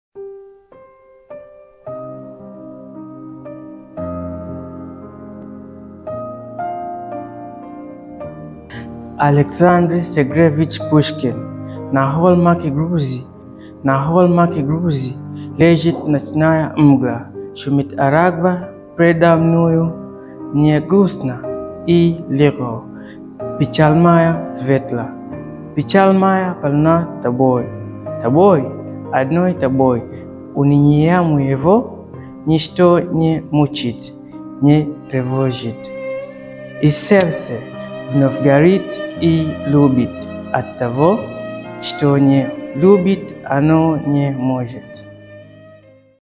На холмах Грузии
Стихотворение "На холмах Грузии"
Читает Салум Нуру